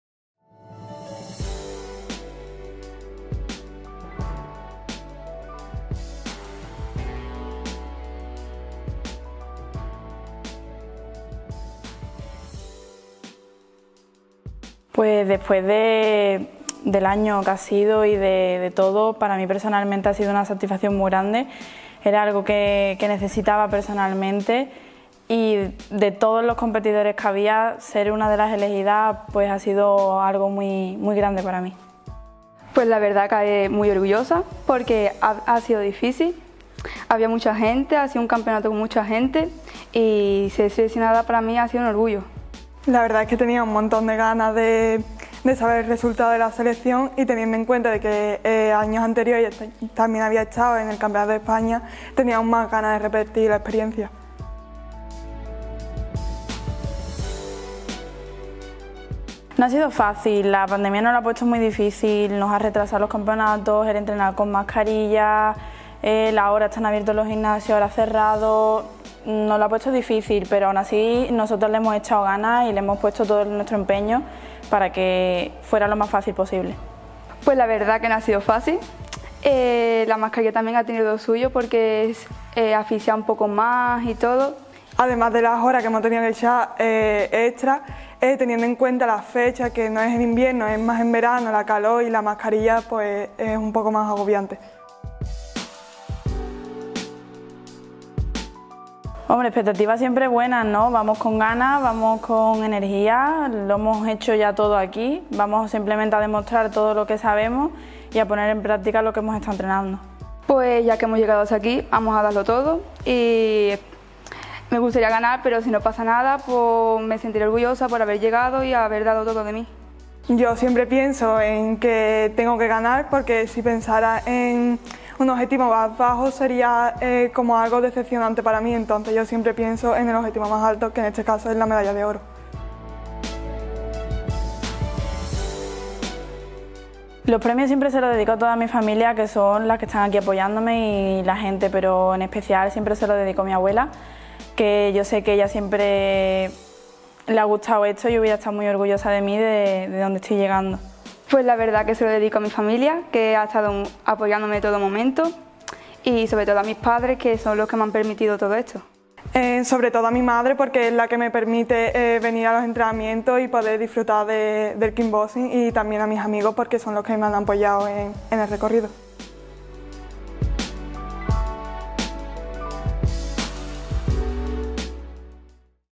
Hablamos con: Tres puertorrealeñas seleccionadas para el Campeonato de España de kick boxing
Hablamos con Ainhoa Colomer Moreno (categoría senior), Sandra Gomar Sánchez (categoría cadete mayor) y Alba Pérez a Ramírez (categoría cadete mayor), tres puertorrealeñas seleccionadas para el Campeonato de España de kick boxing.